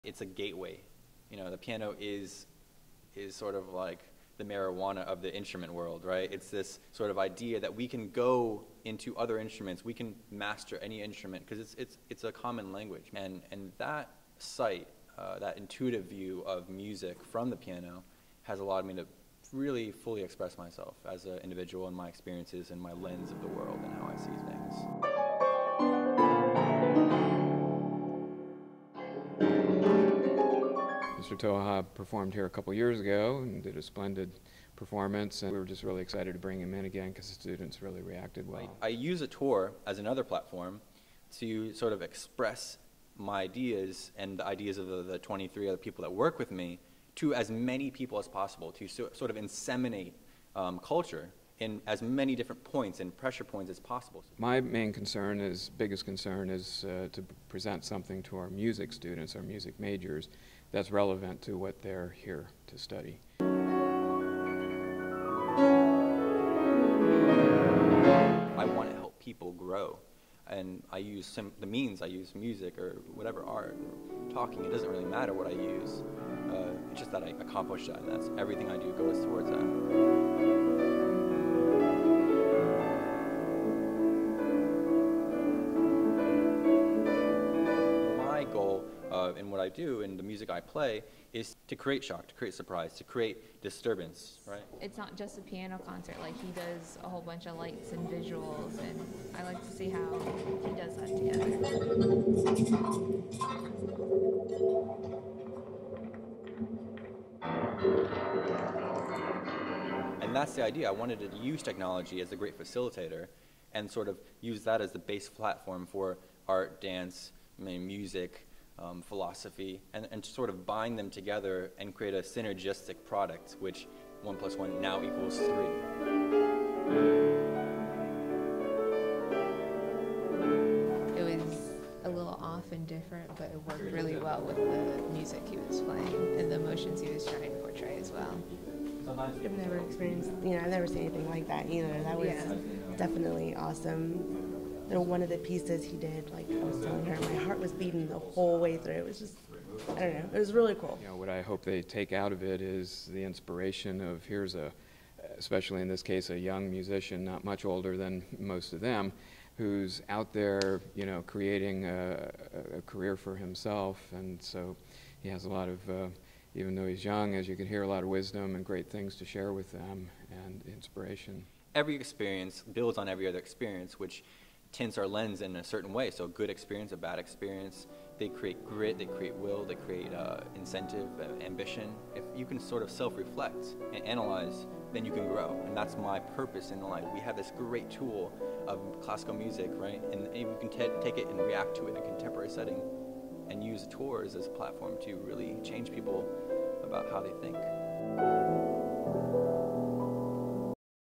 HCC Piano Concert Photo Essay By Hawk TV's Joesph D'Amato
HCC's Ybor City Campus enjoyed a special concert by one special pianist Julian Toha. Check it out!